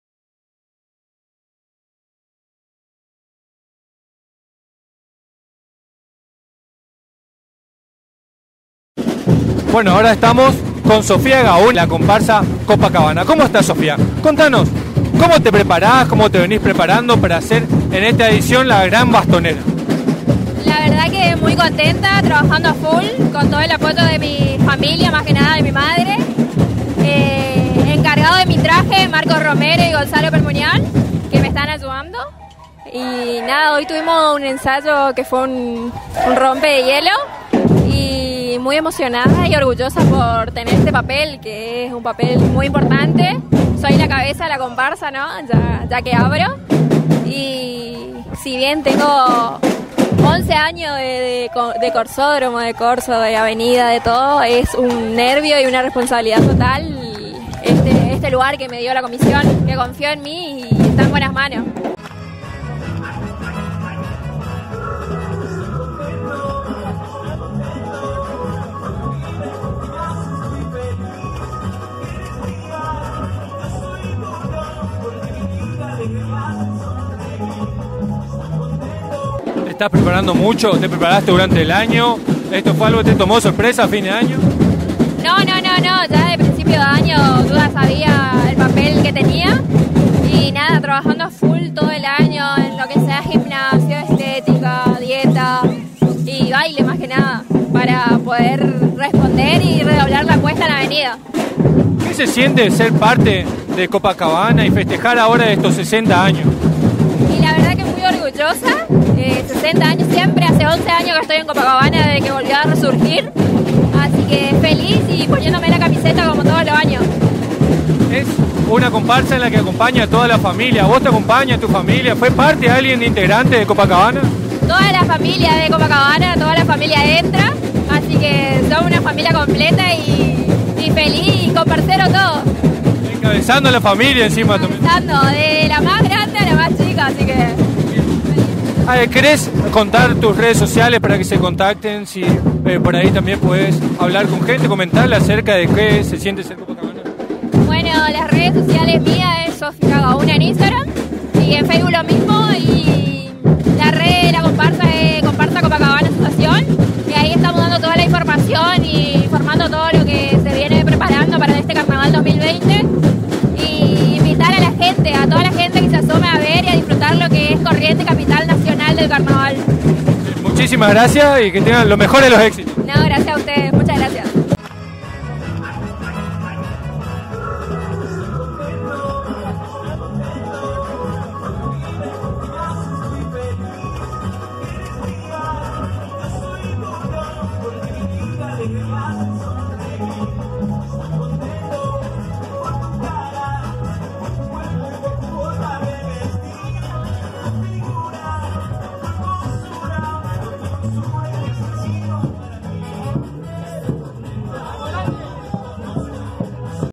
COPACABANA 2020 SOFIA GAUNA FIGURA DE DESTAQUE
ENSAYO COPACABANA, HABLAMOS CON UNA FIGURA DESTACADA SOFIA GAUNA.